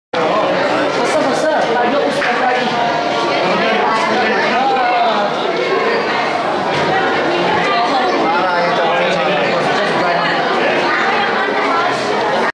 Playful White Tigers
These White Tigers are behind the safety glass at Bali Safari and Marine Park.